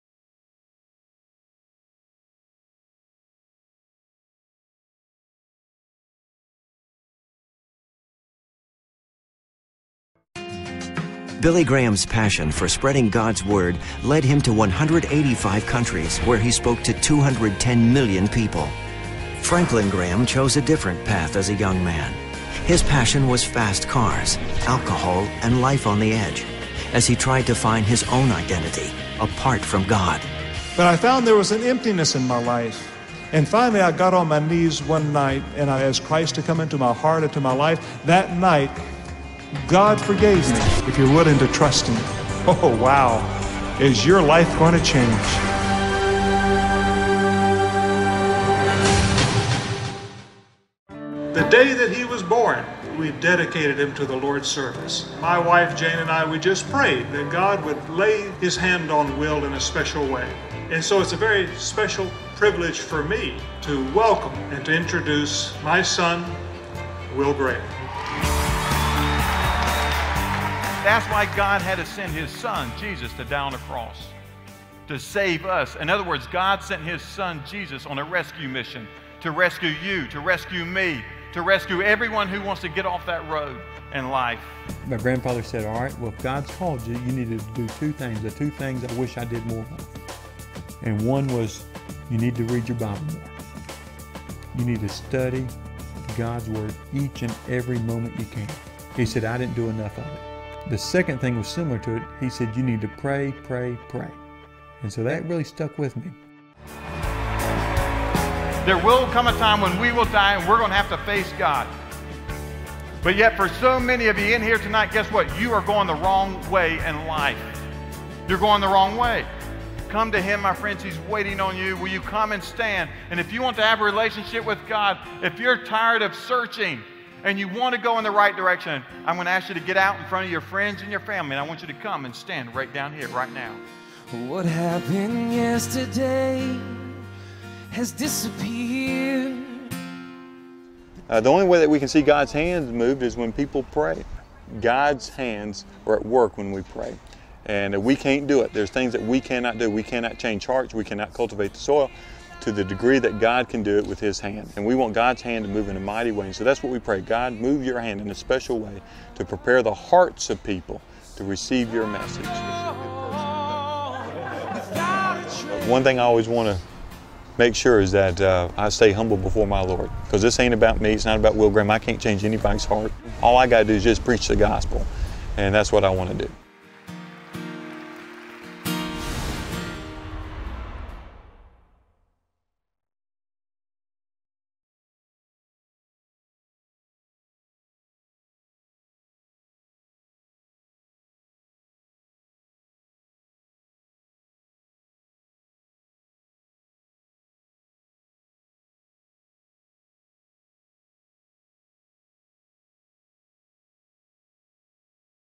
Franklin og Will Graham kommer til Norge i 2016.
Billy Graham Evangelistic Association sender Franklin Graham og hans sønn Will Graham til Norge i 2016. Kontaktperson i Norge er Frank Søgaard.